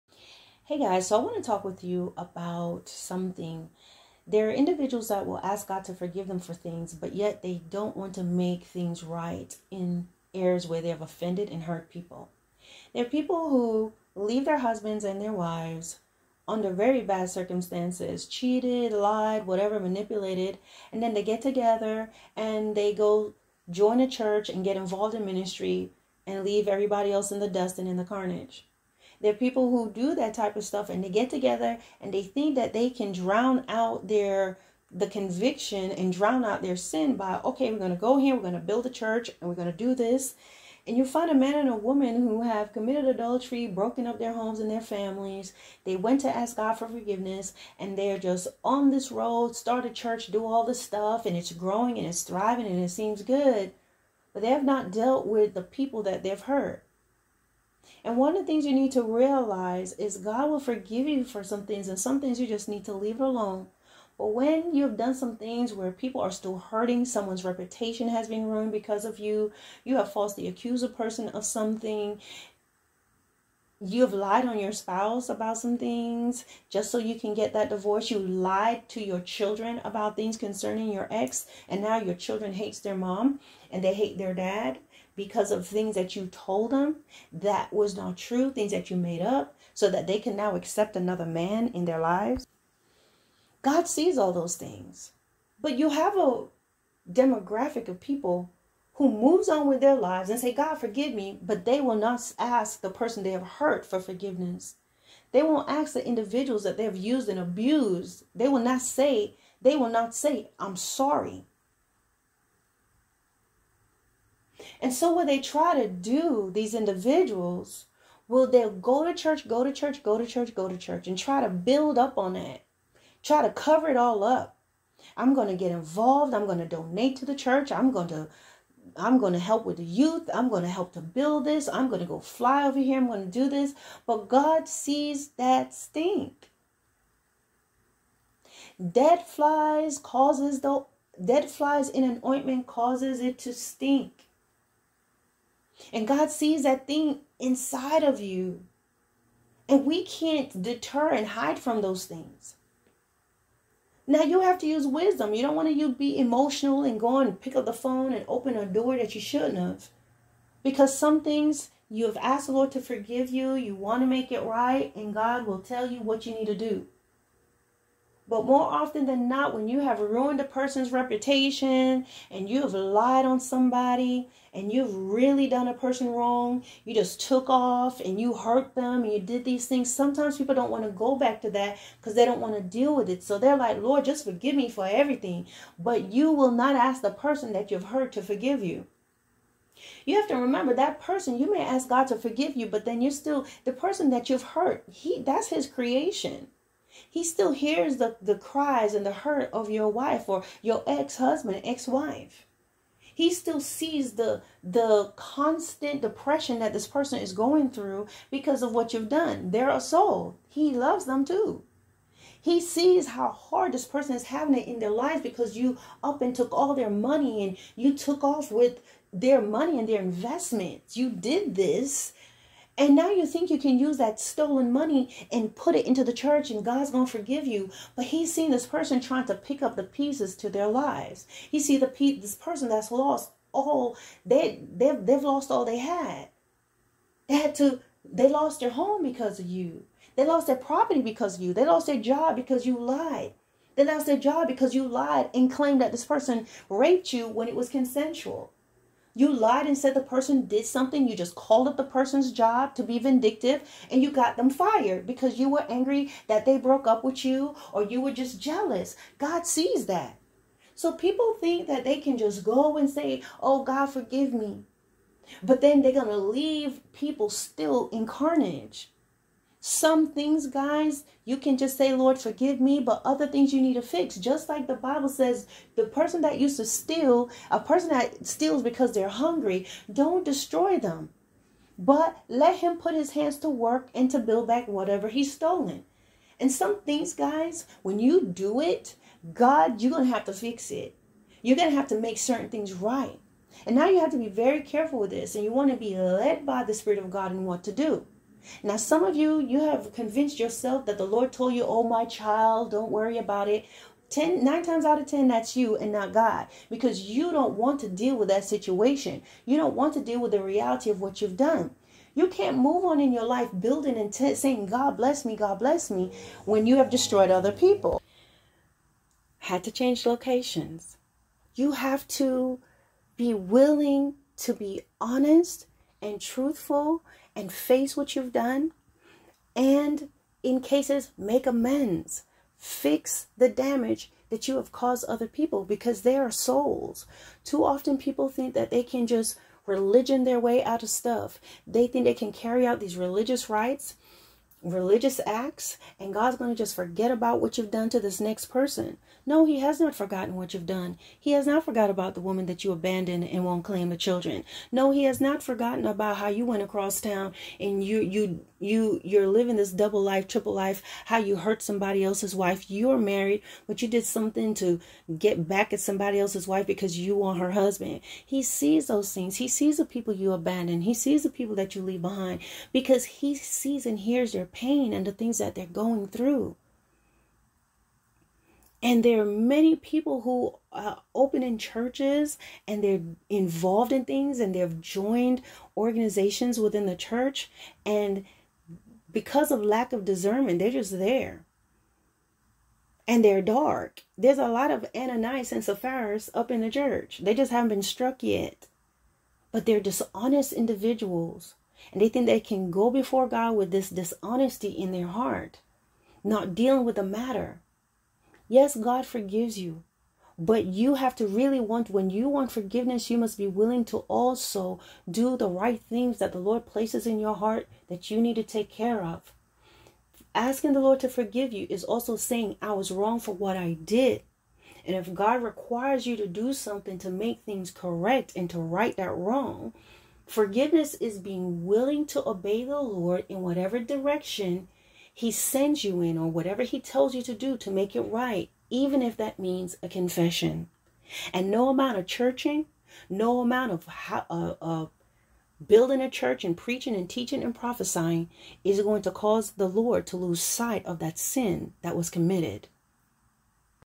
GOD Forgive Me! But Forget Those I've Hurt
The Lord doesn't forget. Forgiveness may still require facing consequences and being accountable.